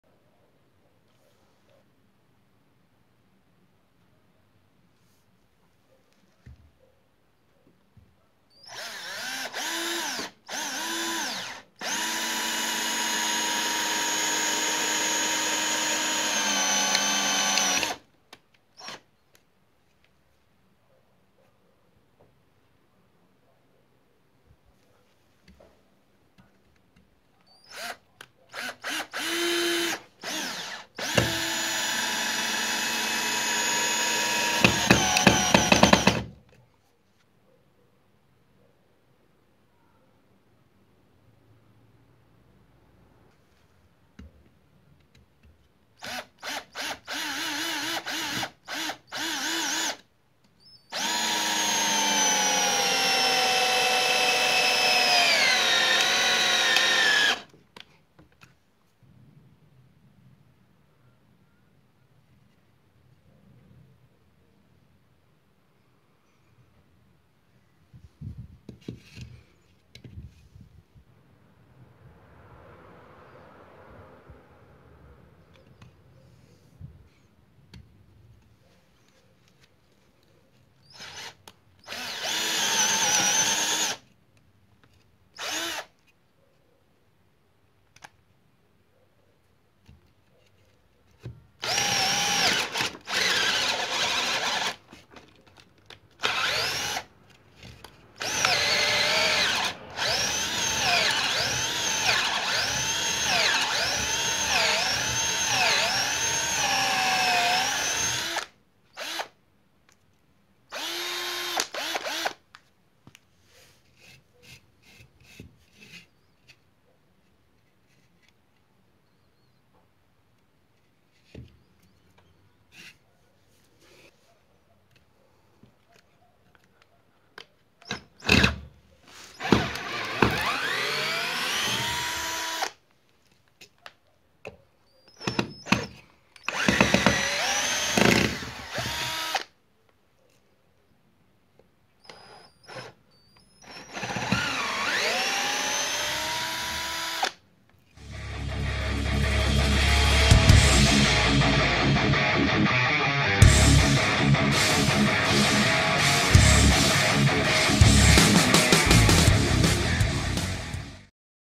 Hitachi DS10DAL Masina de insurubat si gaurit cu acumulator 10,8V
Hitachi DS10DAL a venit in set impreuna cu o lanterna, o valiza de transport speciala de foarte buna calitate, doi acumulatori Li-Io de 1,5Ah si o statie de incarcare. In clip veti vedea un scurt test de insurubare si gaurire in lemn cu aceasta masina de insurubat si gaurit de la Hitachi.

Date tehnice Hitachi DS10DAL:

Capacitate mandrina 0.8 - 10 mm
Tensiune acumulator (V) 10.8
Greutate (kg) 1
Diametru maxim de gaurire in lemn (mm) 29
Turatia in gol ( rot/min ) 0 - 350 / 1300
Alimentare Cu acumulatori
Diametru maxim de gaurire in otel (mm) 10
Capacitate acumulator (Ah) 1.5
Moment de torsiune maxim - insurubare dura (Nm) 36
Moment de torsiune maxim - insurubare moale (Nm) 14
Diametru maxim suruburi 6 mm